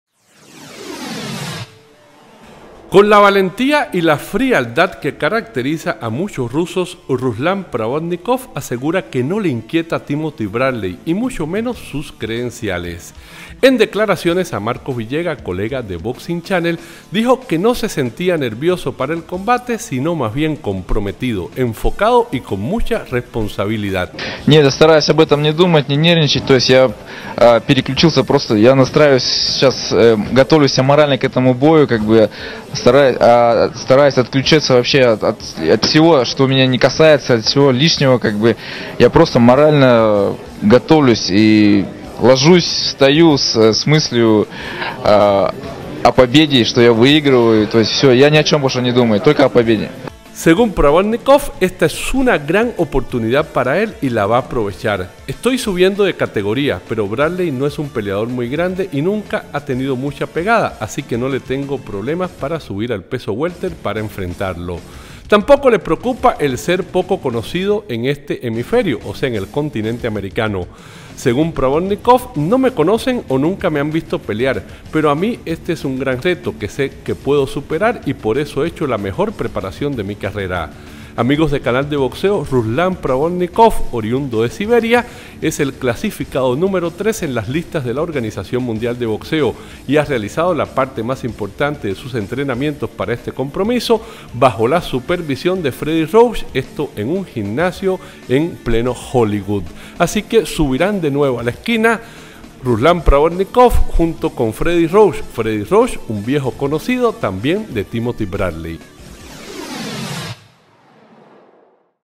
A Ruslan Providnikov no le inquieta Timothy Bradley
Provodnikov ve totalmente viable vencer a Timothy Bradley